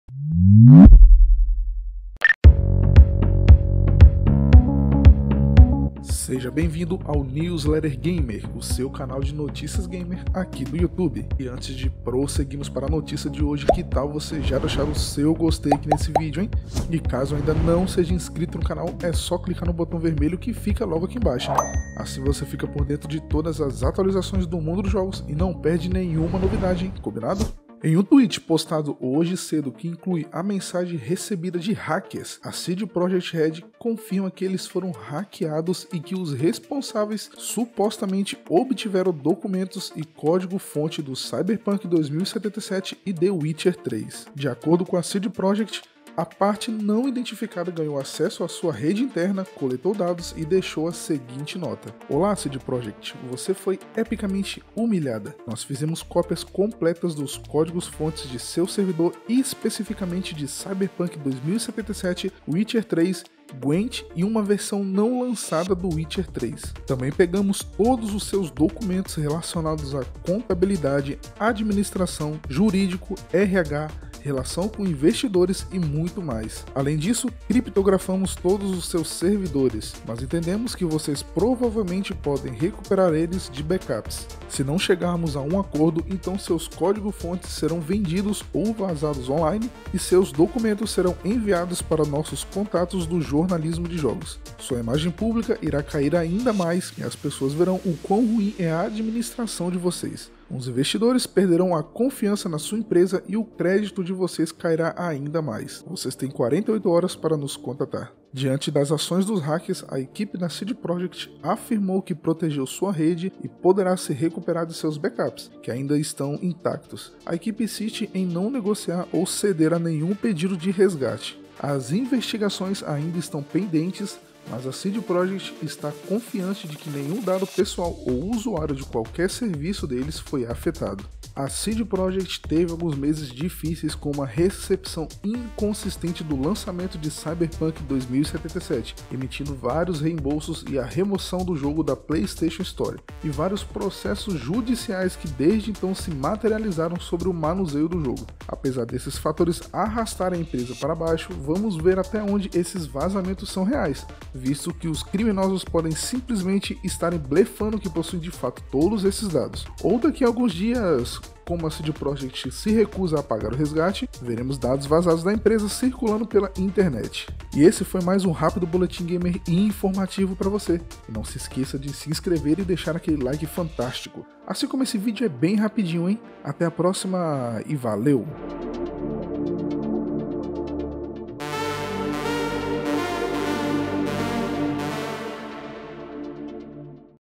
CD Projekt Red sofre ataque hacker! - Boletim Gamer #21
Em um tweet postado hoje cedo que inclui a mensagem recebida de hackers, a CD Projekt Red confirma que eles foram hackeados e que os responsáveis supostamente obtiveram documentos e código-fonte do Cyberpunk 2077 e Witcher 3.